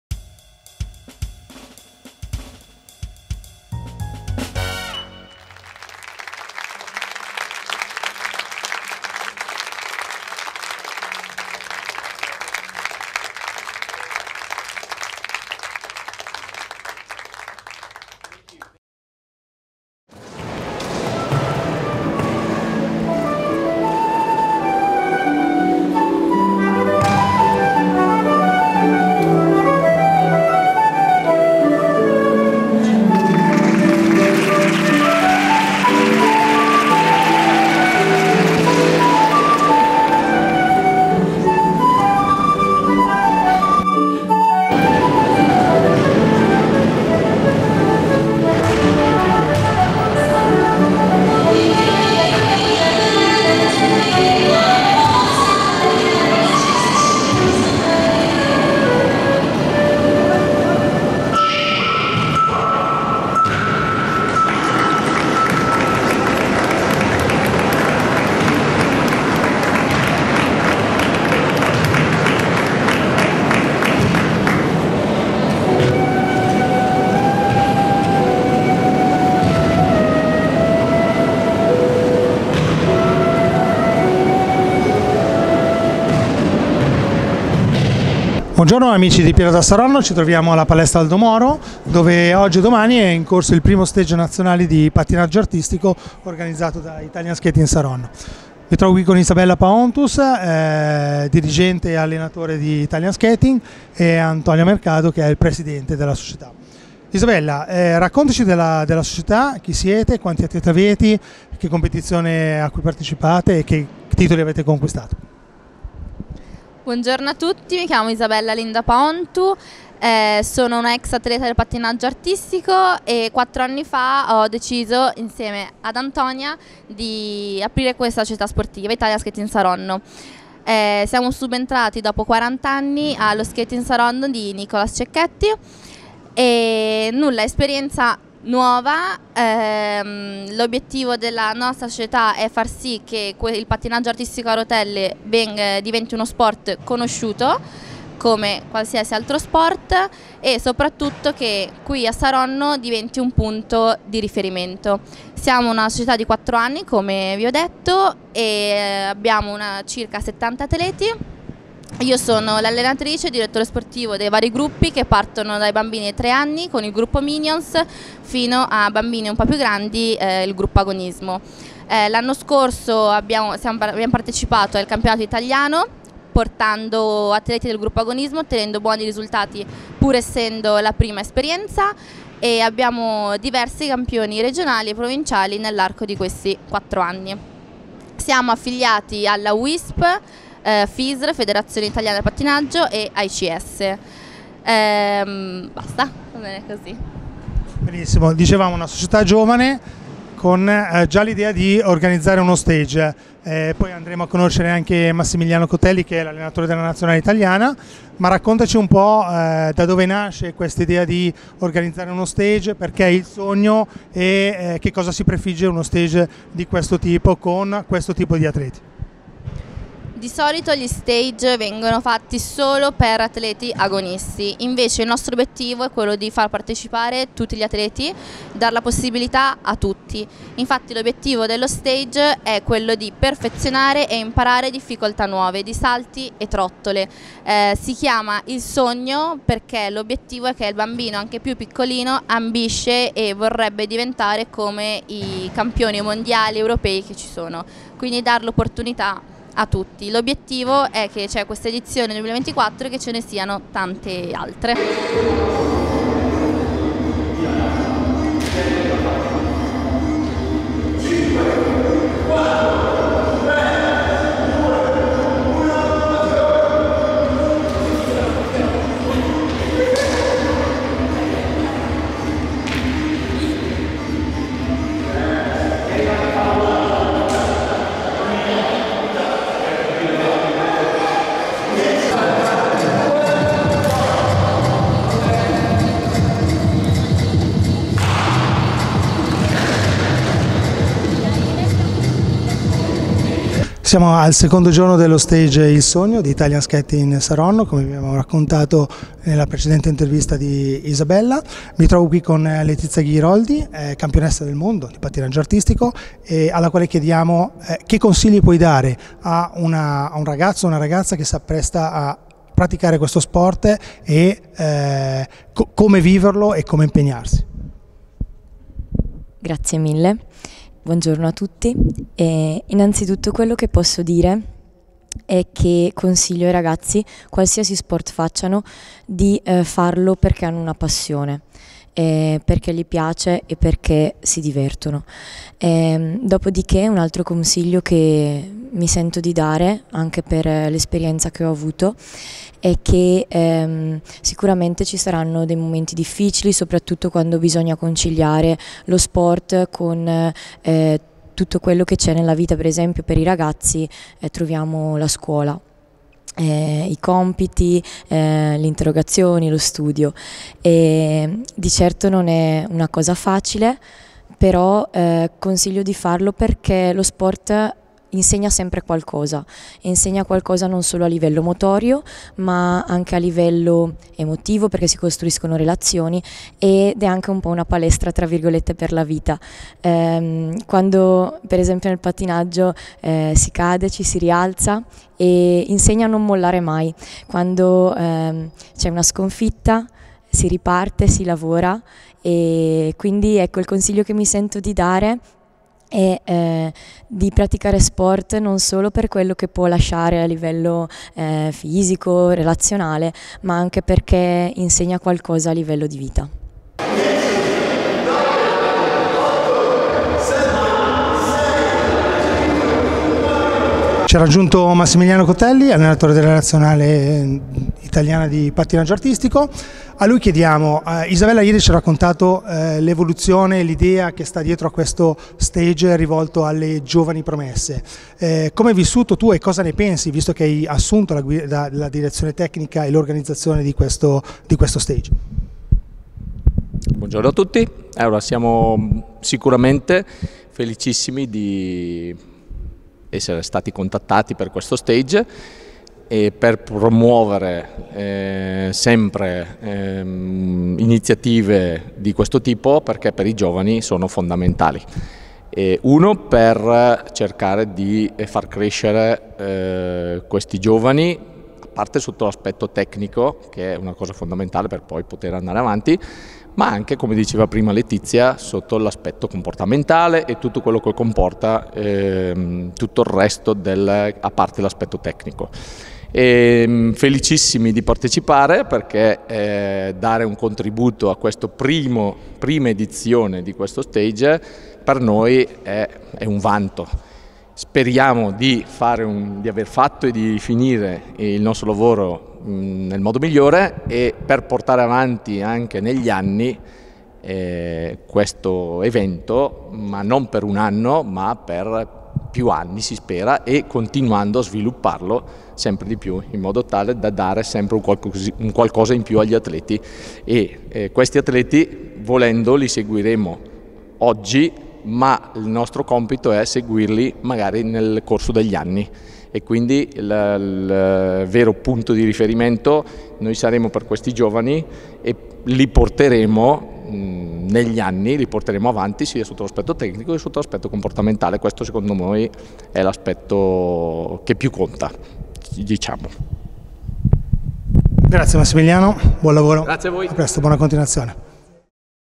Italian skating Saronno "IL SOGNO" - Interviste
Evento a livello nazionale.
Francesco Rancati intervista Isabella Paoountu (allenatrice e fondatrice di Italian Skating Saronno) Massimiliano Cotelli (Allenatore della Nazionale Italiana) e Letizia Ghiroldi (ex atleta della Nazionale Italiana).